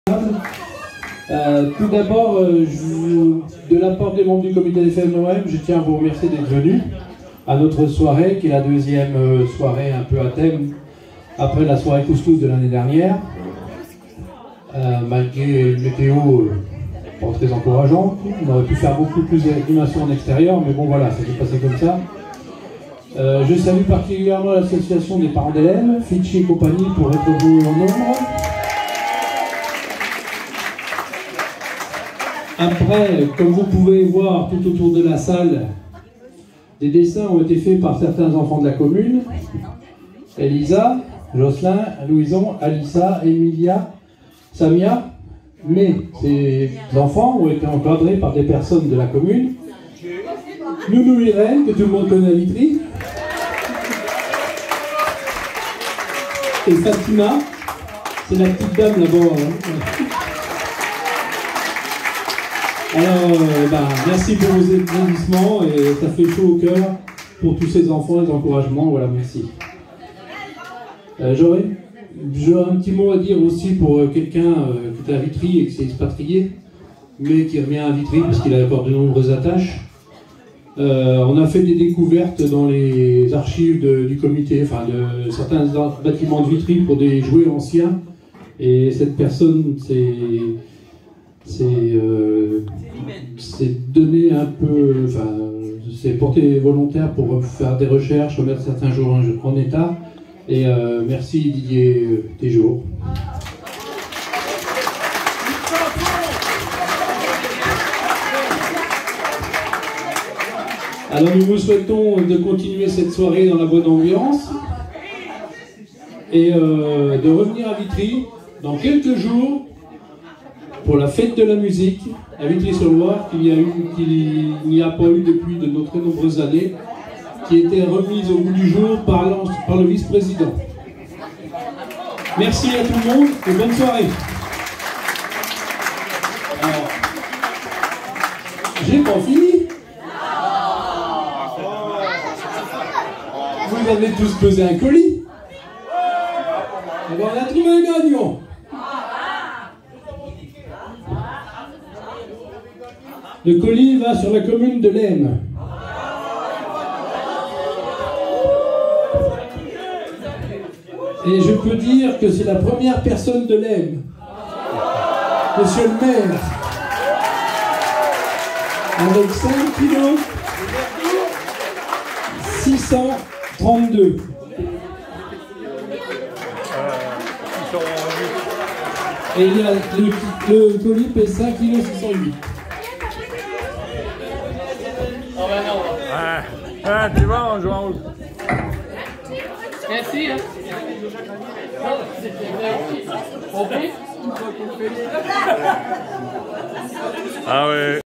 Soirée Paella 2023
Discours du président du comité des fêtes de Vitry sur Loire,  Didier Laurent